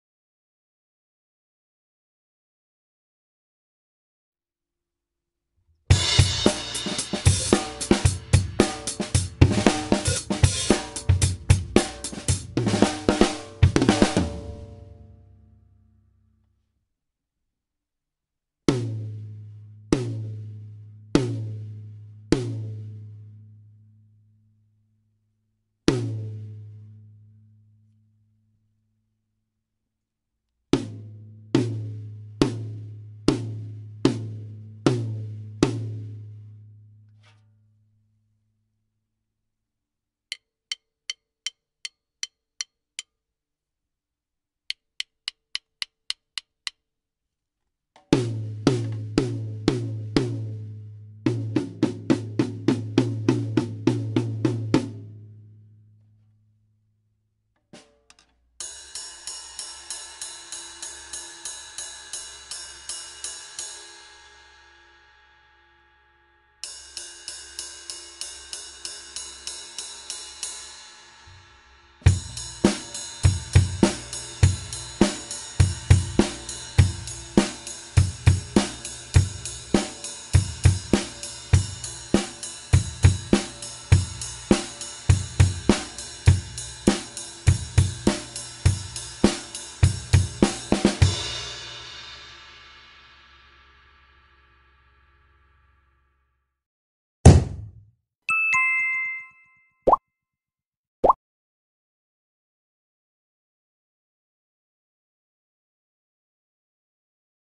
良い音で叩く！
Let's play the drums with a good sound!

HP用の動画です。
詳しい解説や譜面はコチラへ。

more video&tips.